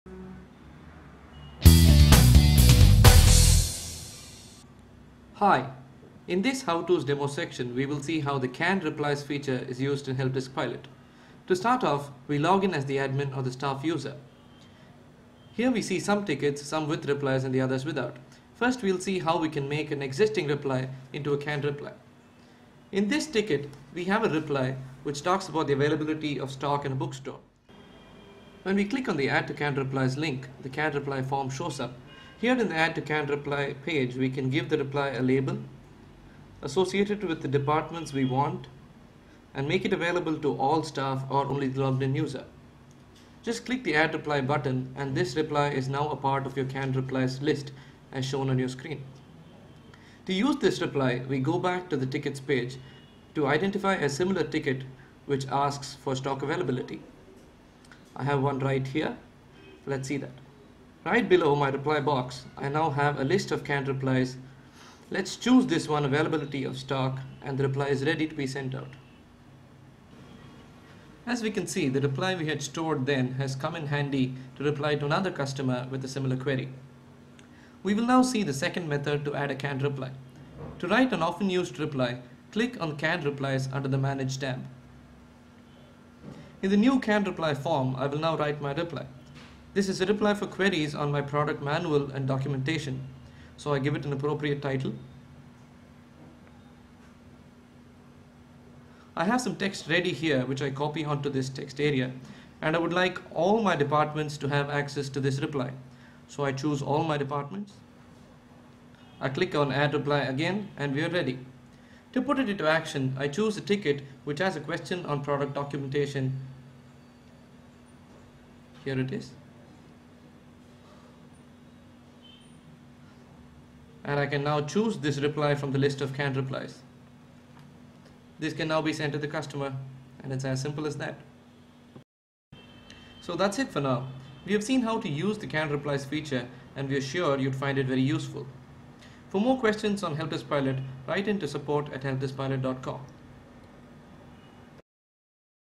Helpdesk Pilot How to Demos-Canned reply
In this episode of Helpdesk Pilot How tos, the Canned reply feature is introduced. How to add and edit canned replies and increase your productivity.